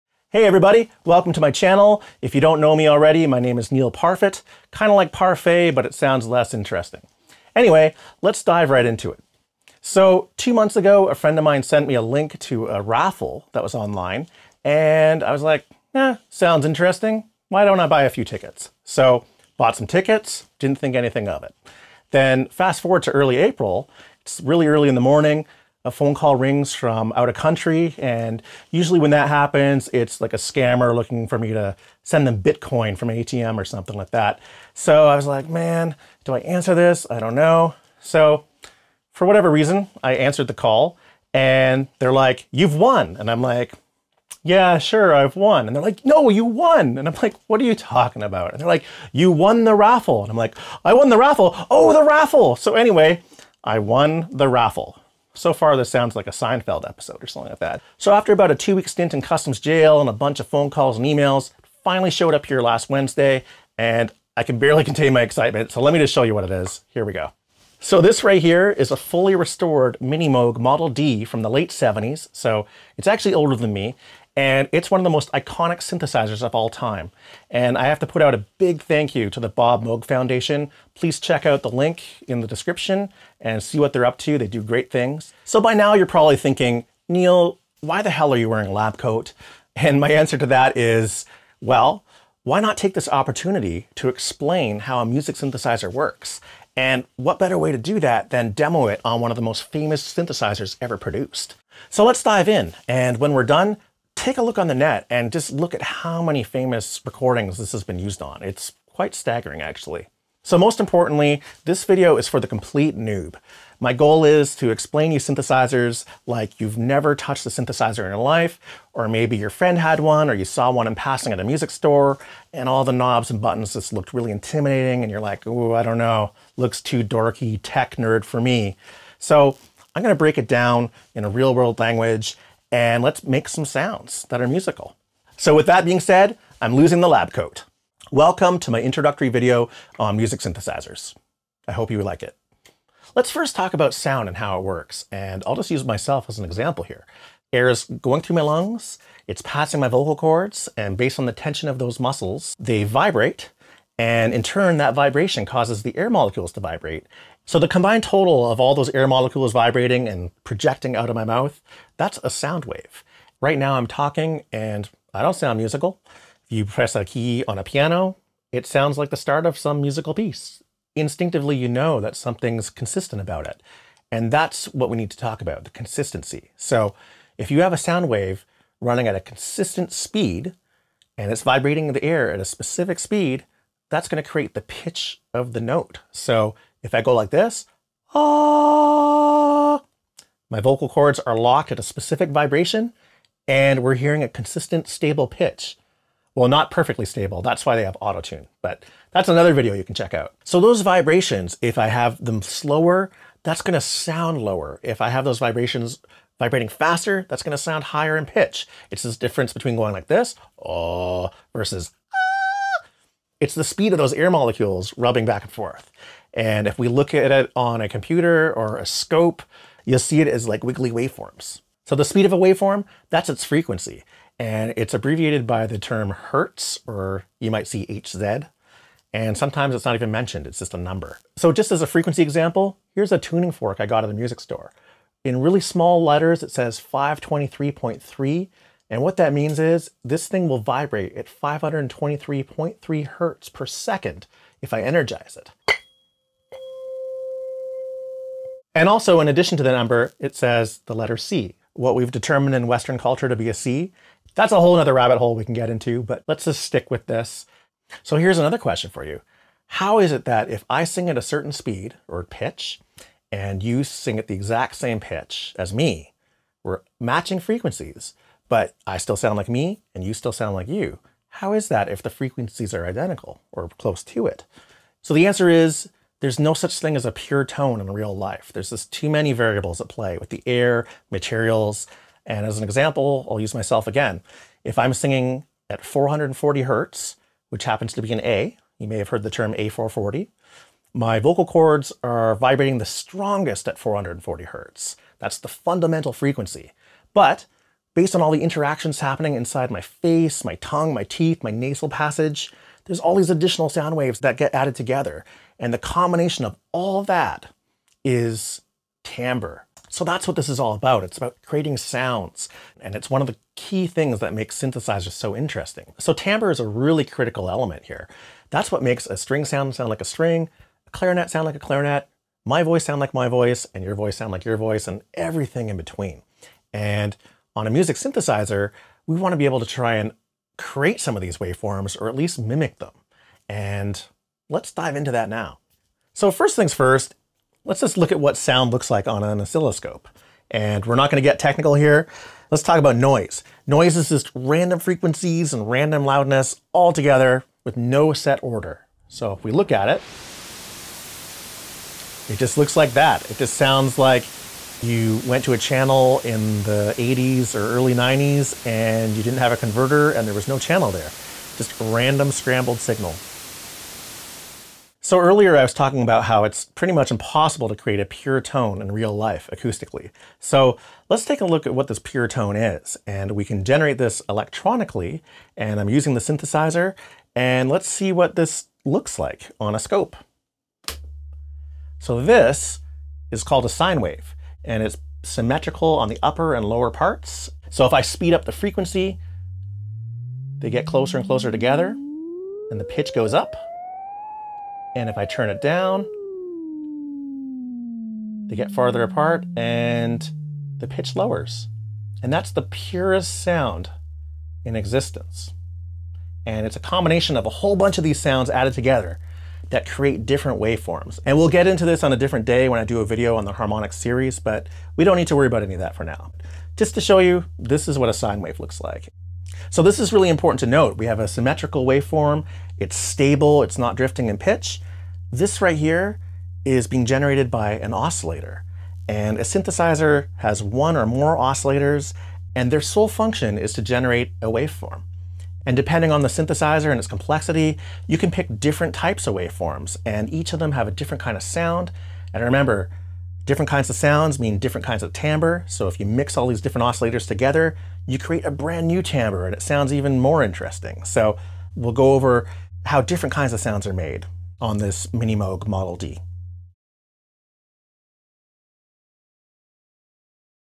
The Ultra Noob/Newb Guide to Music Synthesizers - #1
Let's get non-nerdy!!

But first,  a warning... there is some strobing artifacts starting about 2/3rds through from the scope.  You have sensitivity to flashing lights and such,  I’d recommend a small window vs full screen viewing.

If you've seen a Synthesizer and been all..  "what the hell is this thing?"  "Looks scary with all those buttons and knobs"   "This thing can only be for nerds" ... Look no further!

This is my candid,  casual,  multi-video introduction to music synthesizers for the absolute newb/noob user.  This is not a deep dive.

Come on in knowing nothing,  and walk away knowing something is my goal!  Welcome!

0:00 Forward
1:27 What's with the Labcoat?
1:50 The Goal of this Video Series
2:17 Welcome!
2:57 Pitch / Frequency
4:17 How do two people singing the same pitch still sound different if it’s the same frequency?
5:36 What does sound look like on a Scope?
5:45 I meant to say "Equal" Loudness
6:04 What is the Purest Tone?
7:00 What is an Oscillator?